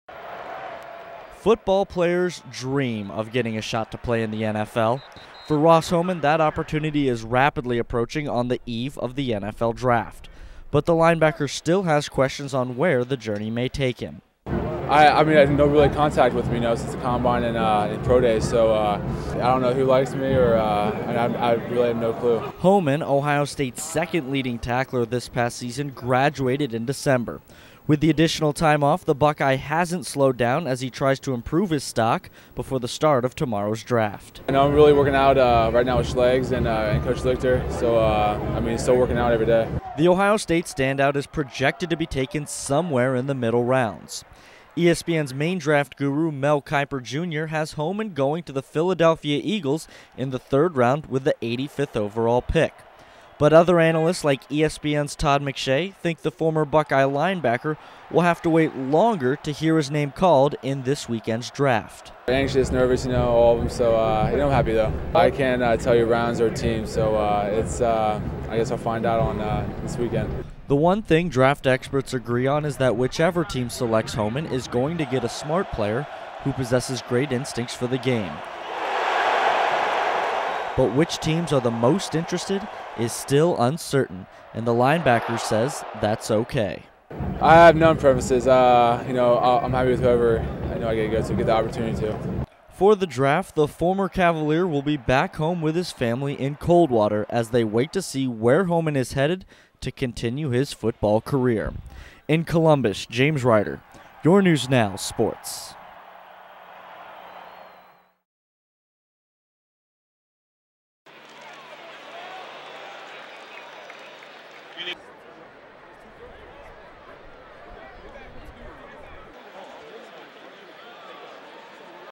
Ross Homan's NFL Draft Prospects
Coldwater's Ross Homan has waited 24 years to reach the NFL, so what's a few more days or hours.  The former Cavalier should be a selection in the 2011 NFL Draft.  But when and where he'll be taken is up in the air.  This is the story previewing Ross Homan heading into the 2011 NFL Draft.

UPDATE: Ross Homan was drafted in the 6th round with the 200th overall pick by the Minnesota Vikings.  He becomes the first player from Coldwater to reach the NFL.